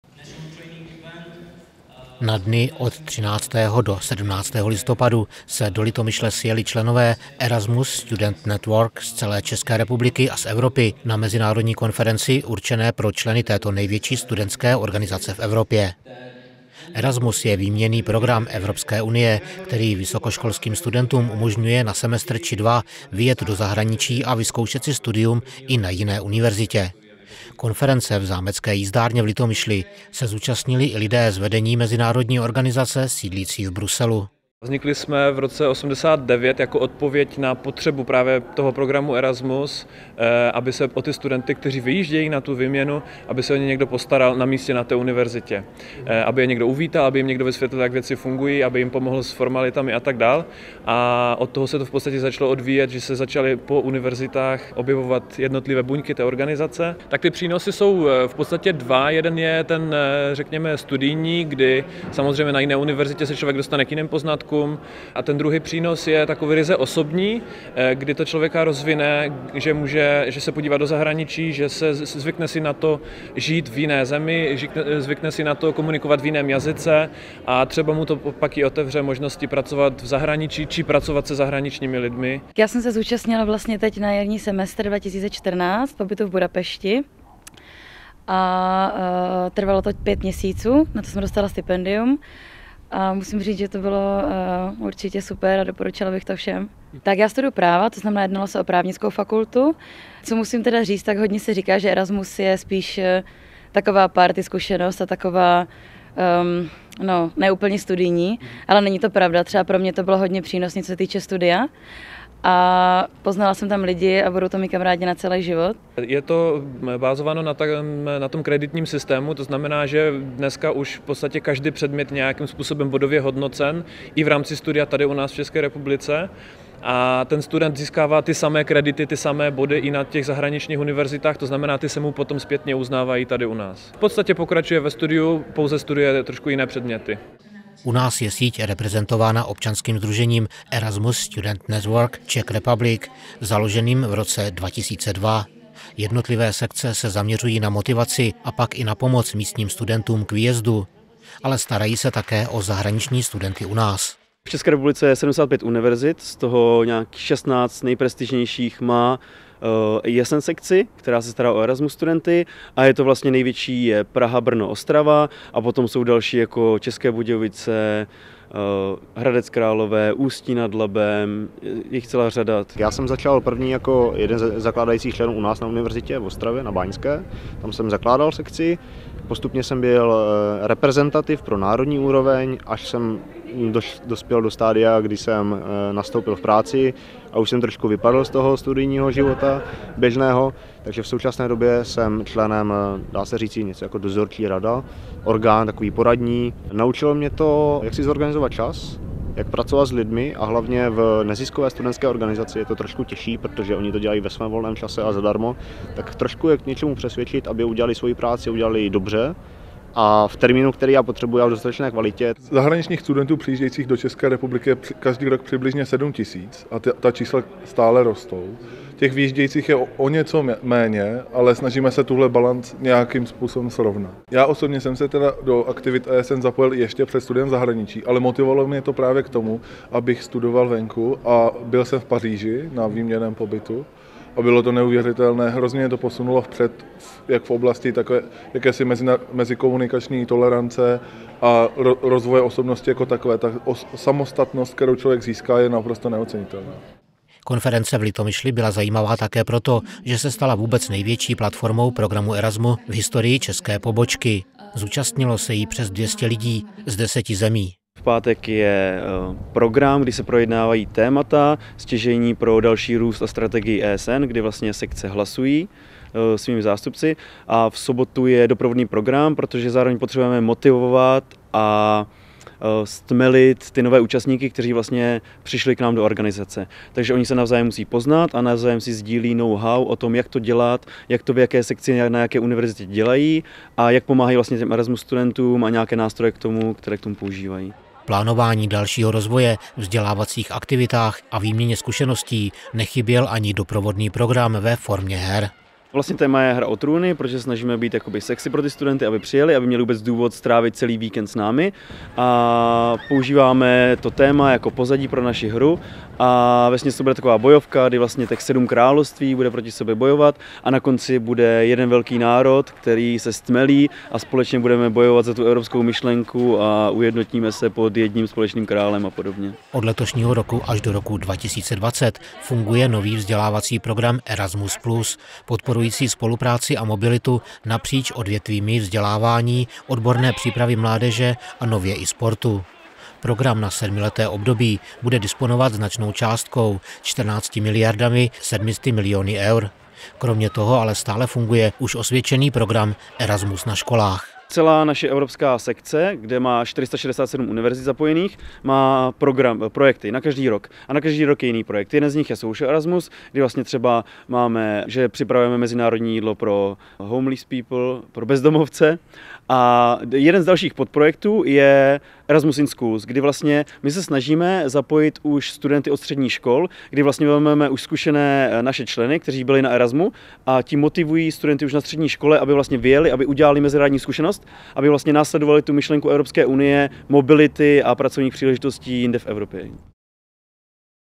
Erasmus - historicky největší konference v Česku
CMS TV Litomyšl 2014

Od 13 do 17 listopadu se do Litomyšle sjeli členové Erasmus Student Network z celé ČR a z Evropy na mezinárodní konferenci určené pro členy největší studentské organizace v Evropě.